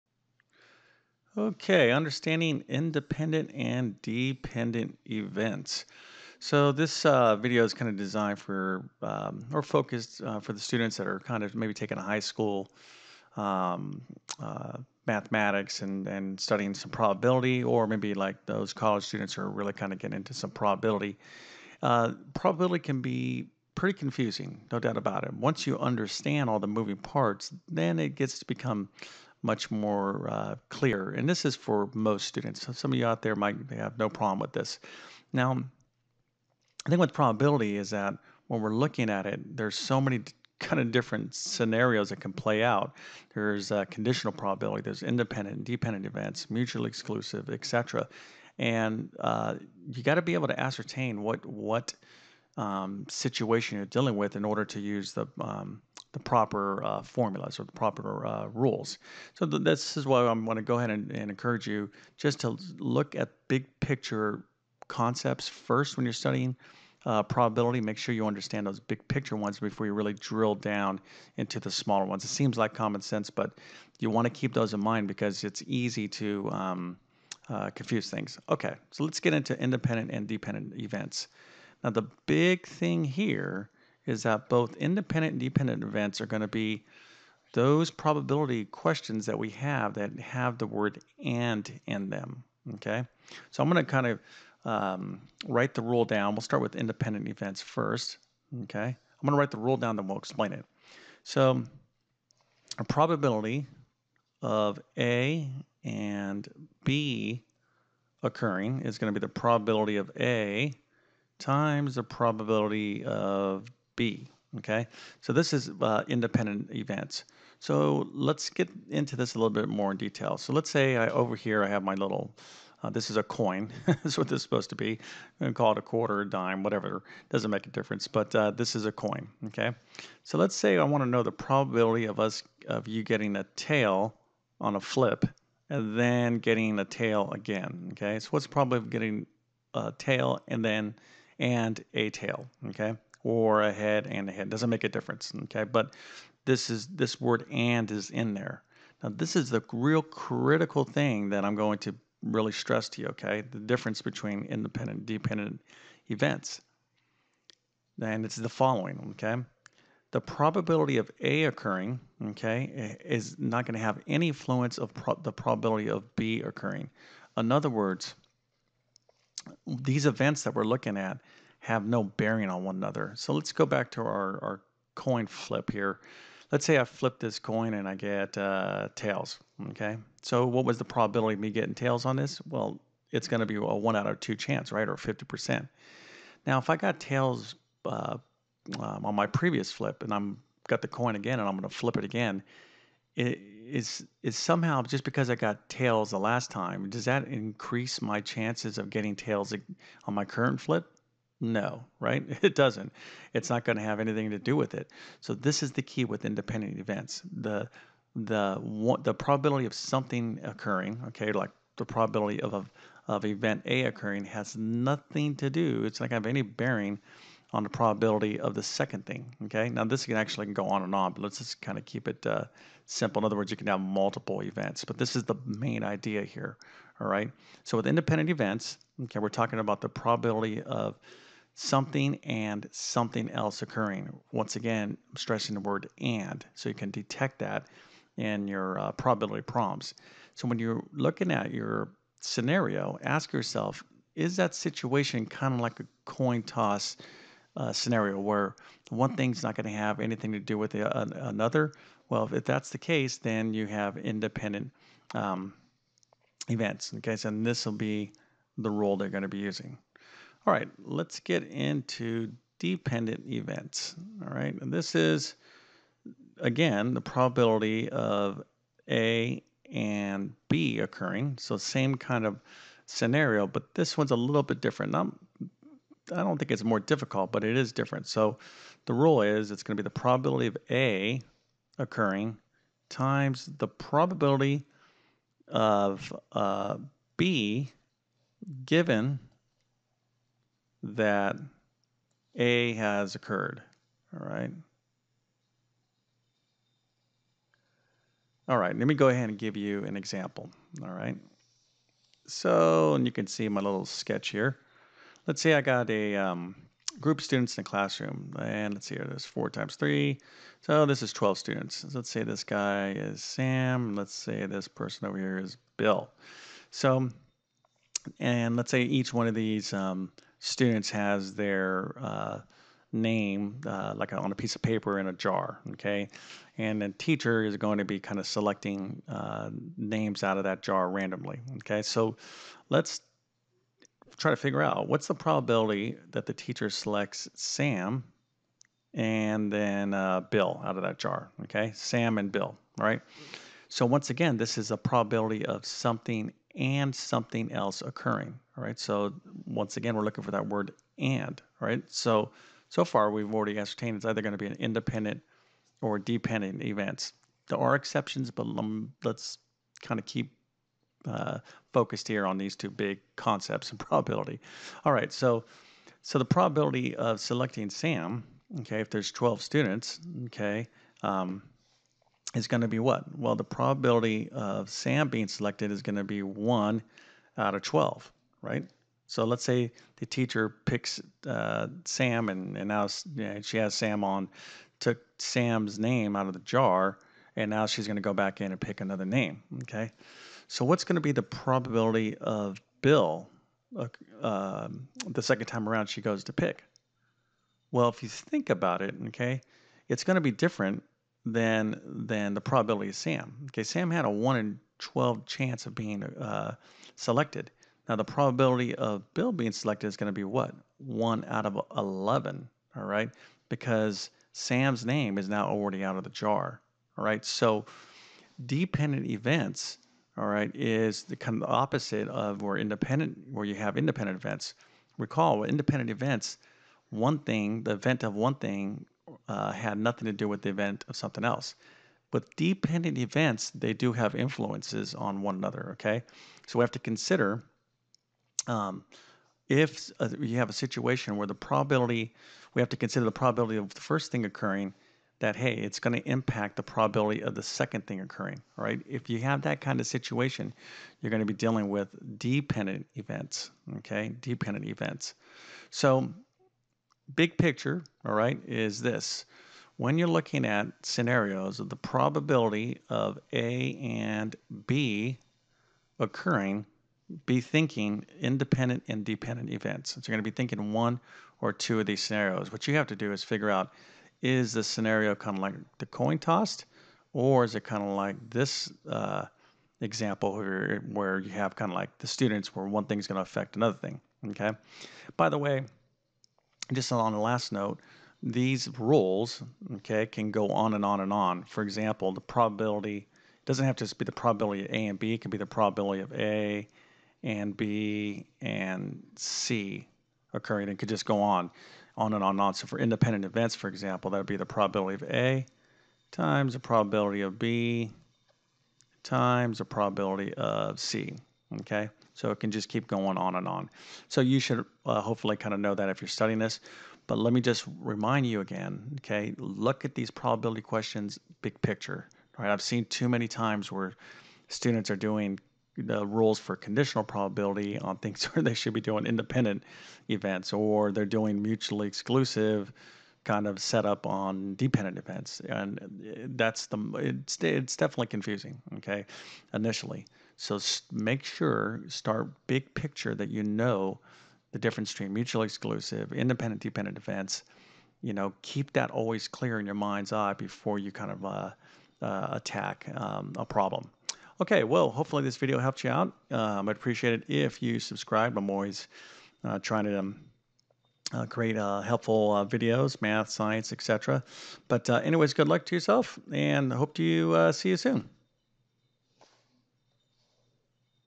Independent and Dependent Events (really understand this stuff)
This video will explain the differences between independent and dependent events.  Most students need help understanding basic probability so this video explains the basics.  For more videos like this please visit TabletClass Math